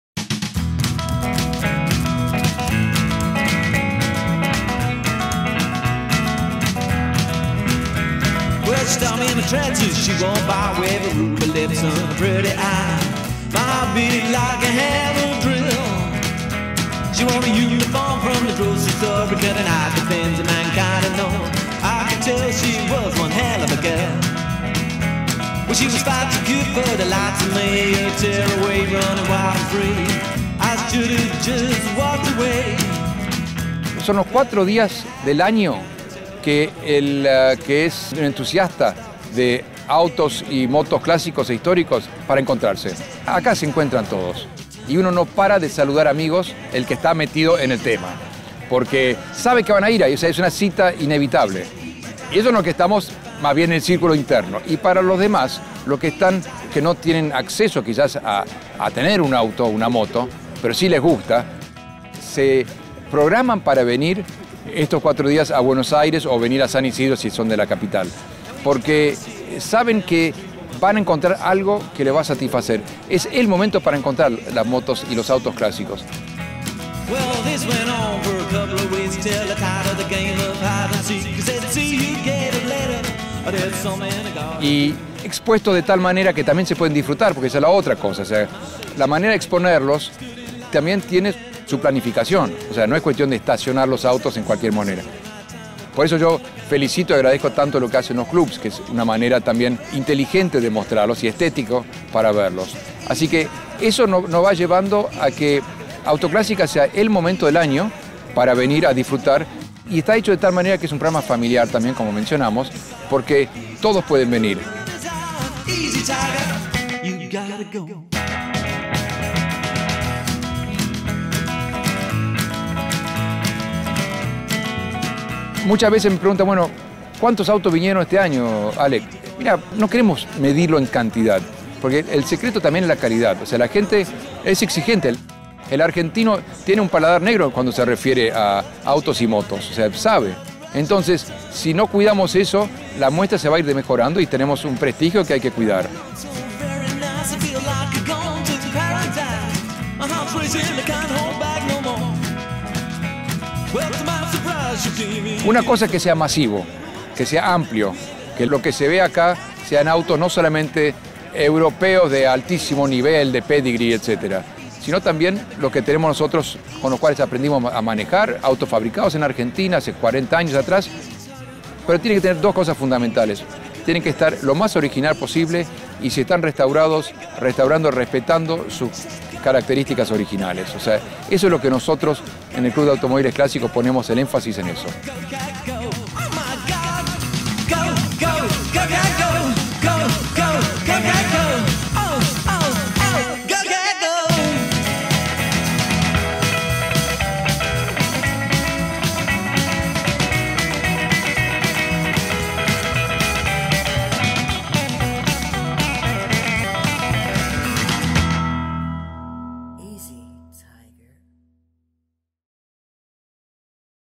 Autoclasica 2014: Best of Show
Más autos clásicos y antiguos y el gran cierre de Autoclásica 2014 con el “Best of show”.
Mini cooper, Ford Mustang, Corvette, Ferrari, Mercedes Benz, Renault, Jeeps Militares, MG y el espectacular Avions Voisin C28.